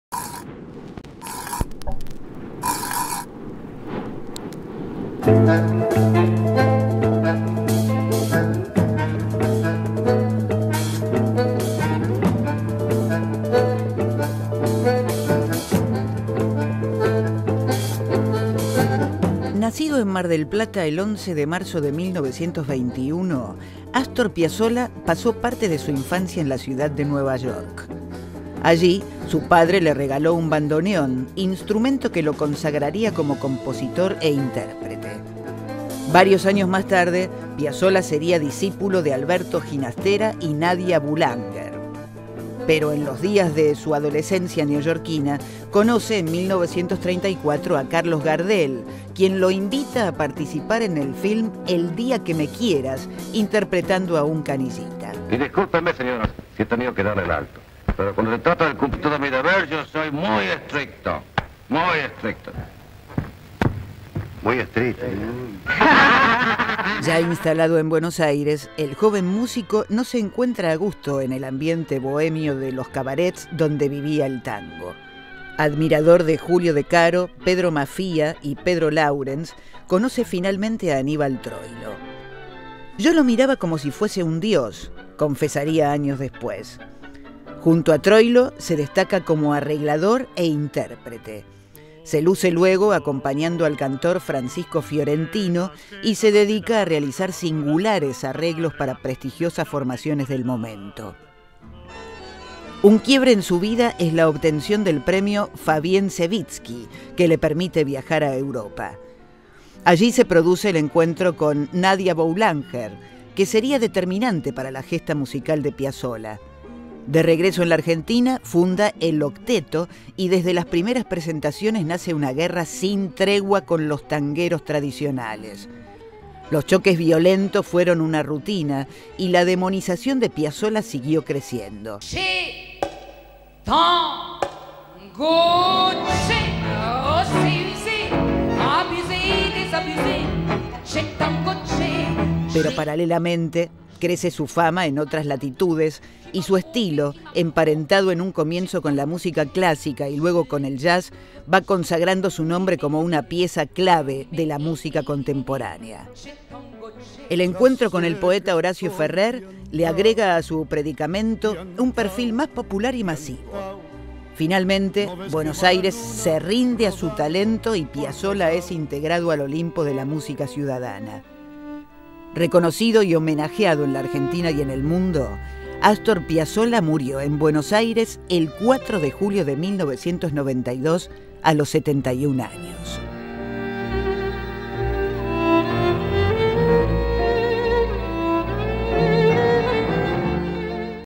HistoriaAlDía​ | 11 de marzo: Nacimiento de Astor Piazzolla
El 11 de marzo de 1921 nace Astor Piazolla en Mar del Plata, Provincia de Buenos Aires.
Su incursión en el tango es un antes y un después en el género. Piazzolla es uno de los referentes más importantes de la música argentina y del siglo XX.

Historia al Día presenta en micro-programas sucesos de la historia argentina y latinoamericana con la narración de distintas personalidades invitadas y la dirección de Alberto Lecchi.